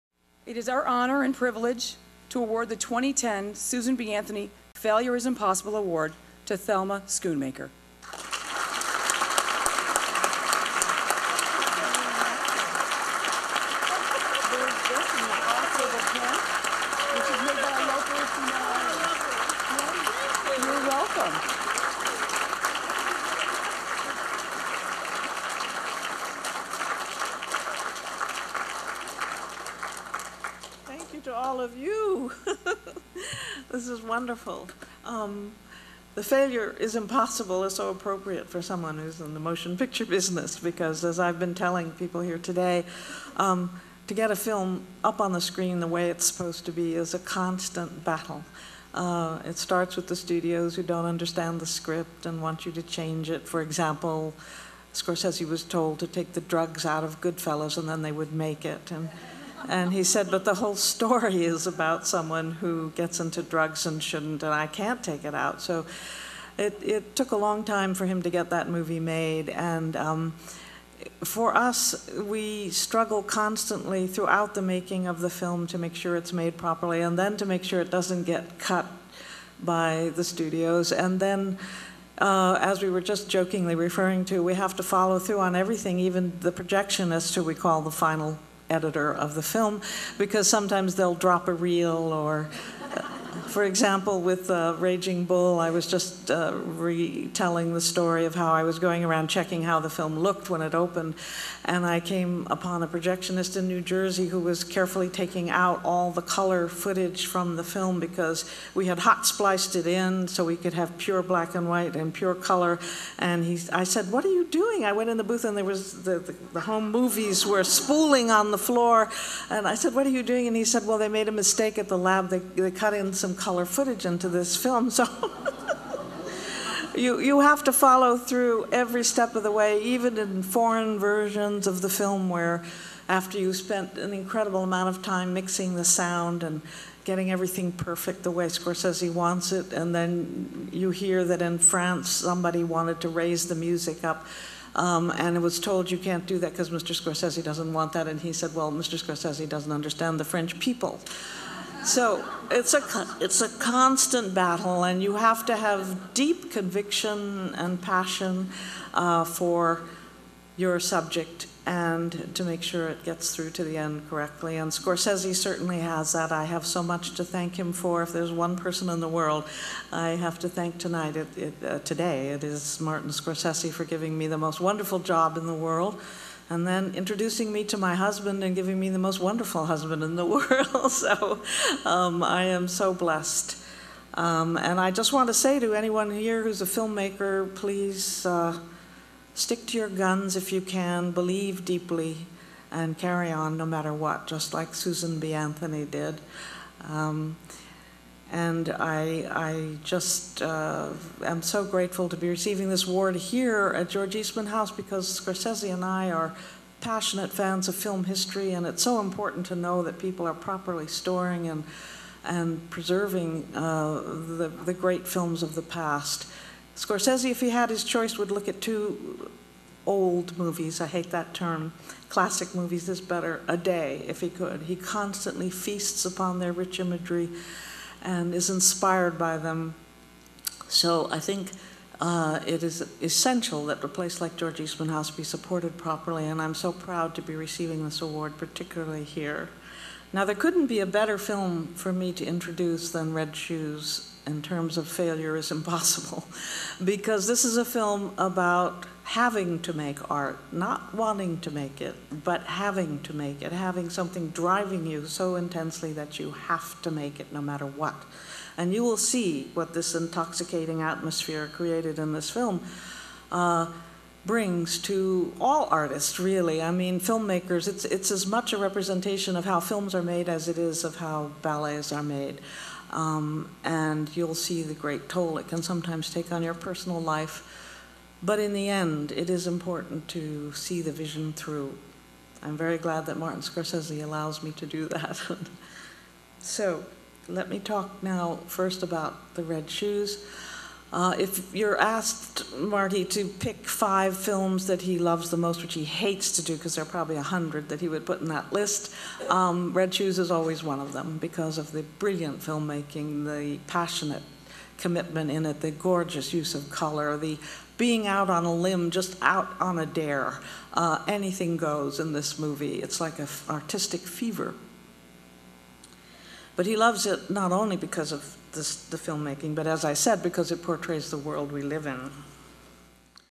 Thelma Schoonmaker at George Eastman House (1 of 2)
The three time Oscar winner has edited every Martin Scorsese film since RAGING BULL.  Here she is in the Dryden Theatre at George Eastman House (Rochester, NY) introducing the restoration of her late husband Michael Powell's film THE RED SHOES.  She is also accepting the Susan B. Anthony Failure is Impossible Award.